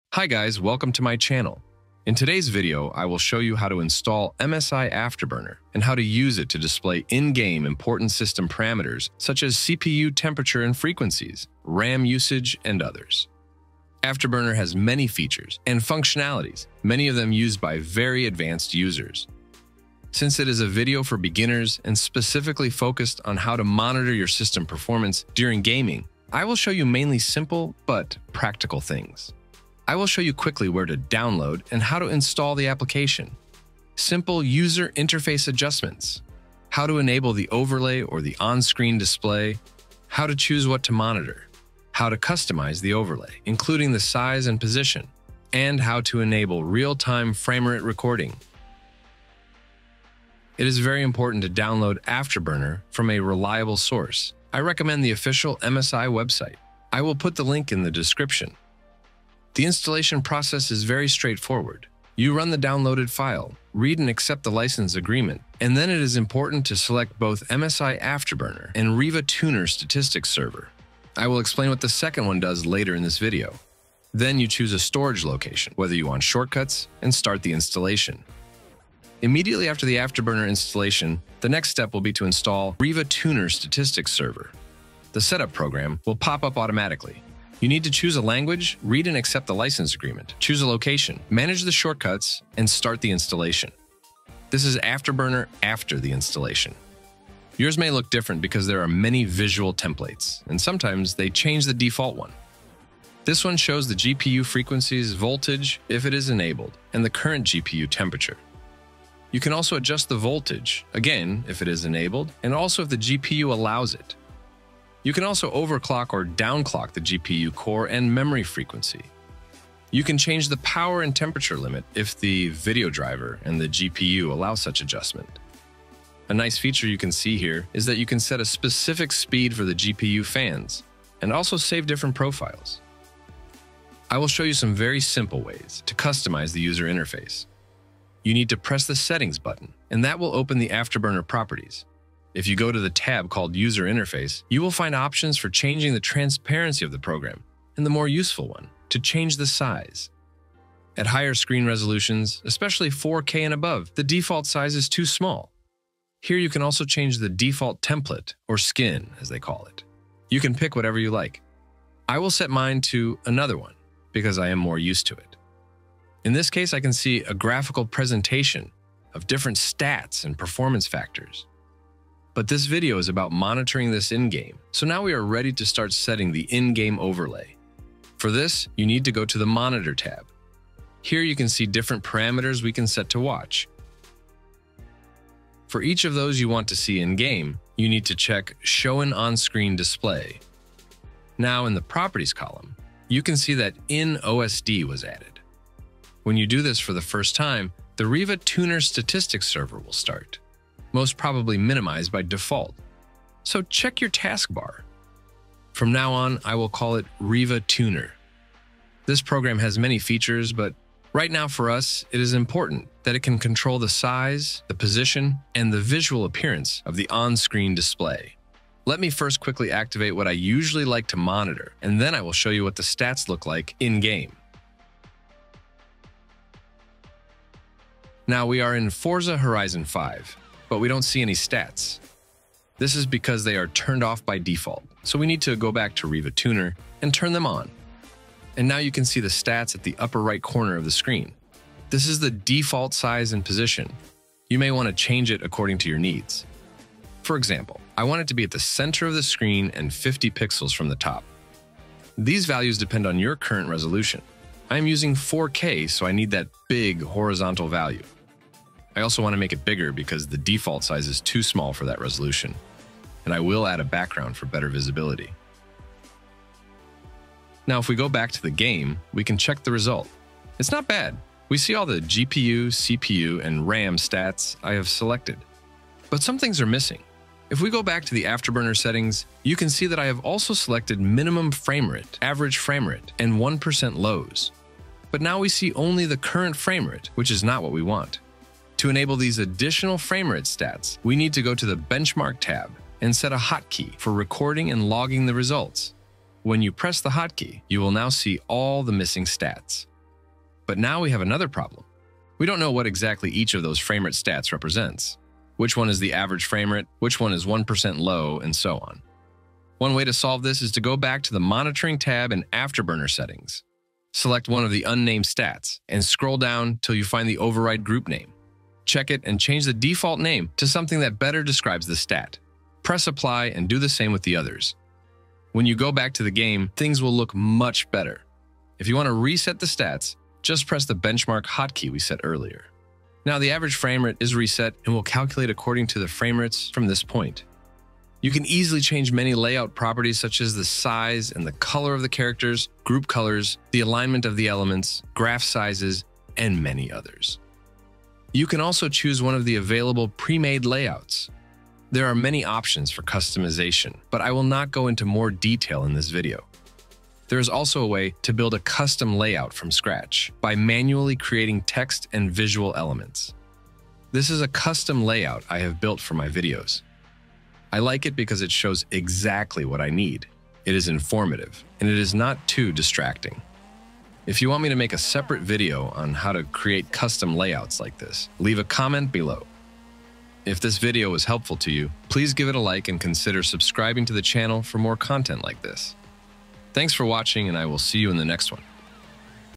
How to Setup MSI Afterburner & In-game Overlay (On-Screen Display) 2024 | Show FPS, temps and speed
How to Setup MSI Afterburner & In-game overlay (On-Screen Display) 2024

In this video I explain how to download and install Afterburner, how to enable the in-game overlay (also called on-Screen display) and how to monitor different system parameters during gaming such as CPU and GPU temps, CPU and GPU frequencies, current fps (framerate), average fps, RAM usage and many others.